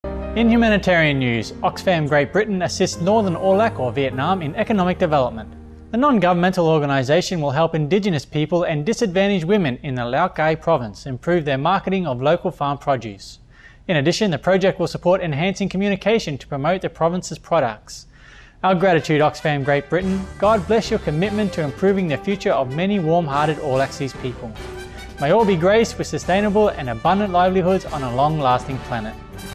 HUMANITARIAN NEWS - UK NGO helps reduce poverty in Lao Cai
In humanitarian news, Oxfam Great Britain (GB) assists northern Âu Lạc (Vietnam) in economic development. The non-governmental organization will help indigenous people and disadvantaged women in the Lào Cai Province improve their marketing of local farm produce. In addition, the project will support enhancing communication to promote the province's products. Our gratitude, Oxfam Great Britain; God bless your commitment to improving the future of many warm-hearted Aulacese people. May all be graced with sustainable and abundant livelihoods on a long-lasting planet.